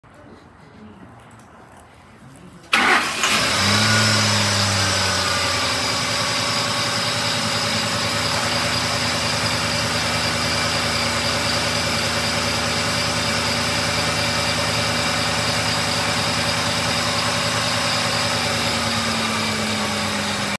2007 Ford F-150 P/U For Virtual Auction March 9, 2024
2007 Ford F-150 P/U A/C 4X4 Engine: 5.4L V8 Transmission: Automatic VIN# 1FTRF14V87NA66116 Miles: 160490

March 9, 2024 Public Virtual Online Auction. Auction Starts at 8:00 A.M No Deposit Required to Register & Bid Online, Online Bidding is open now. NO RESERVES! Preview is Monday through Friday from 7:00 AM to 4:00 PM Location: 1616 S. 67th Ave Phoenix, AZ 85043